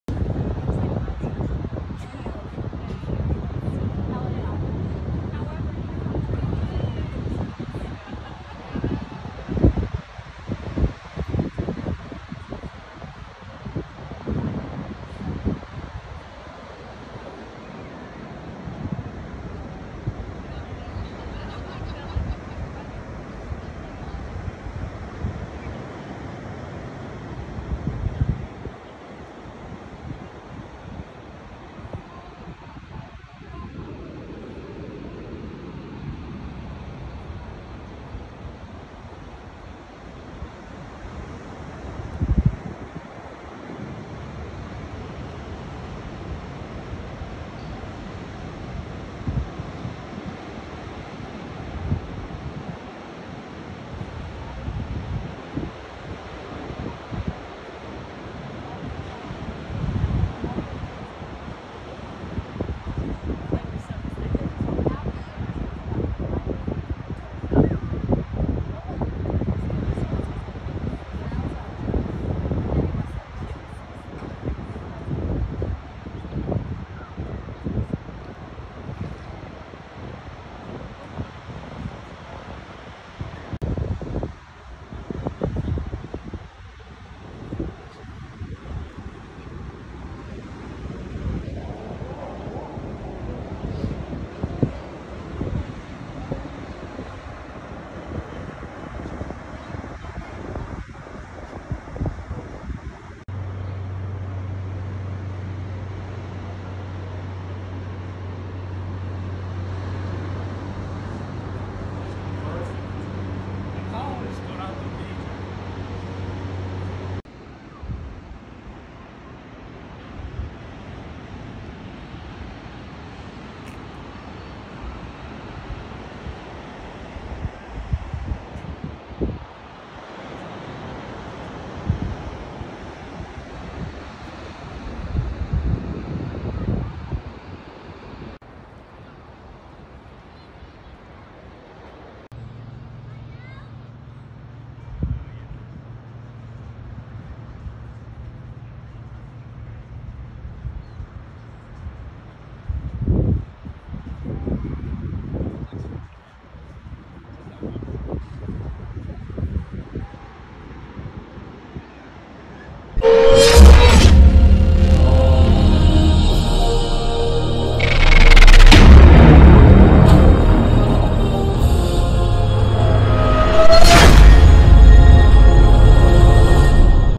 Ritz Carlton resort view in Dana Point , California
The Ritz-Carlton, Laguna Niguel, a resort in Dana Point, California situated on a seaside overlooking the Pacific Ocean, and offering a relaxed Southern California vibe near Laguna Beach.